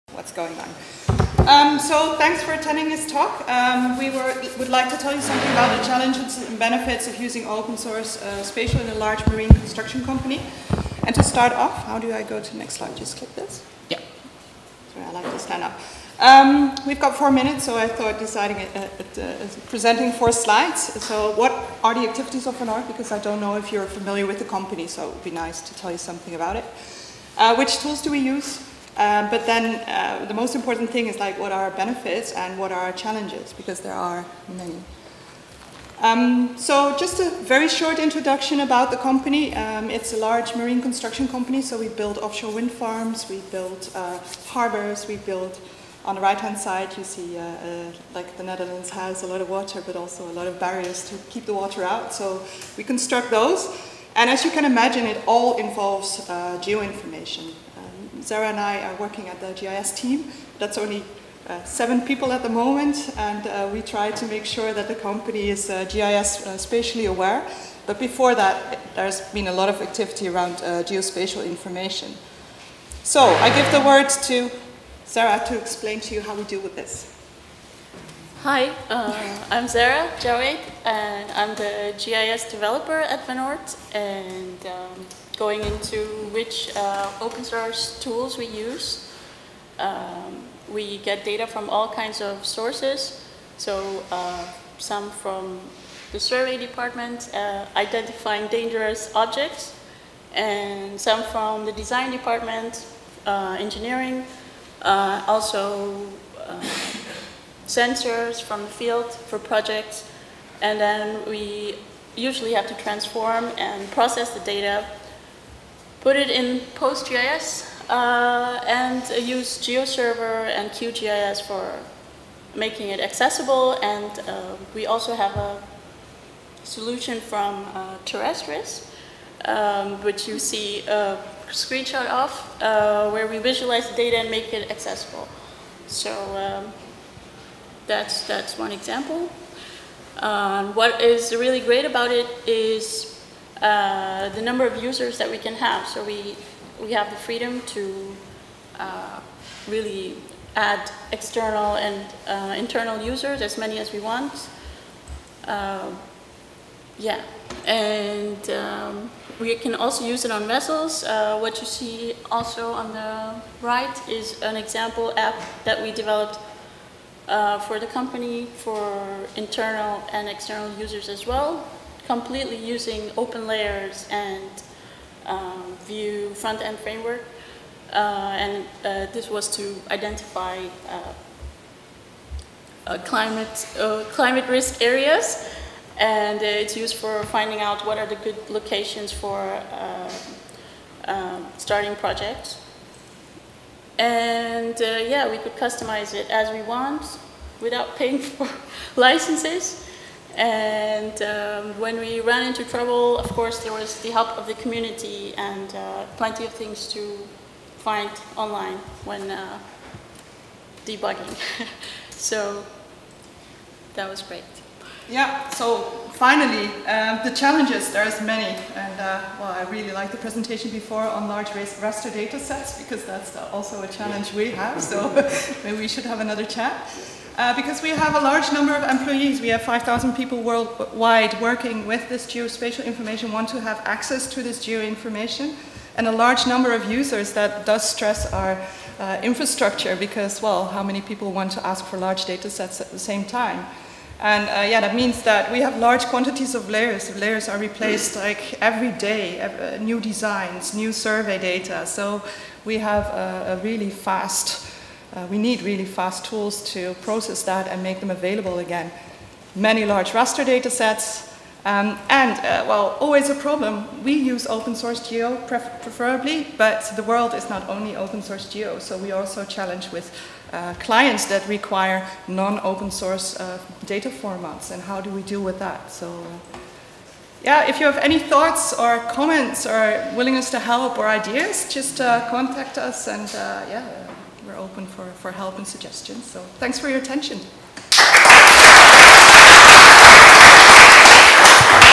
FOSS4G 2022 | The challenges and benefits of implementing OS geo within a large contracting company
Within large construction companies GIS is widely used to store, analyse and visualise spatial data. GIS is just one of the components of an information infrastructure and has to be an integral part that can provide data and information to other departments and subcontractors as well. Van Oord as a marine contractor deals with spatial data on a daily basis. Existing disciplines like the design team and Survey team already have their way of dealing with spatial data to fulfil their task. What is it that the organisation needs additionally to work with GIS?
Van Oord has chosen to use FOSS4g software as a GIS backbone. This choice has its benefits but also poses challenges. Most competitors in the business use proprietary software. Clients do not necessarily follow standards and the IT department has to support our requirements.
We are very proud of our GIS backbone and definitely see the benefits of using FOSS4G. Come and listen to the solution we have chosen; the changes we make in the business and challenges we face.

Irene Pleizier